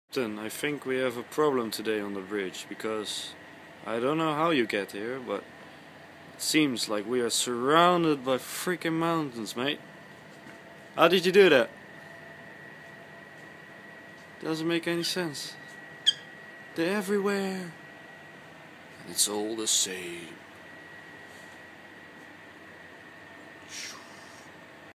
Simulator Fail Failure MSTC Terschelling
While preparing for a exercise at the Full Mission Simulator of the MIWB Terschelling the simulator was not working properly and gave us this awesome view!